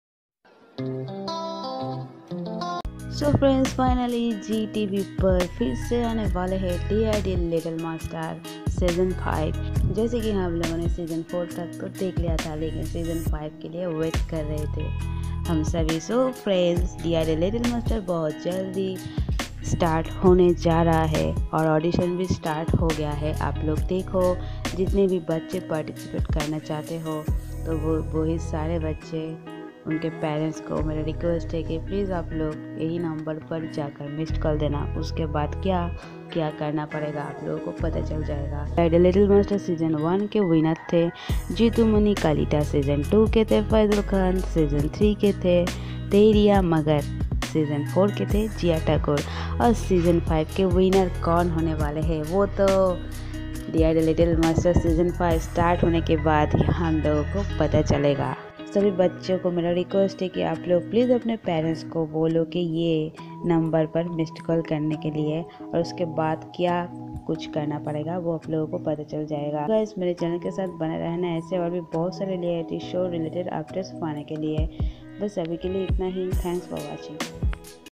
DID little Master Season 5 | Dance India Dance Little Master Season 5 Audition Details
Ids video me DID Little MAster Season 5 ke Audition ke bareme bataya geya he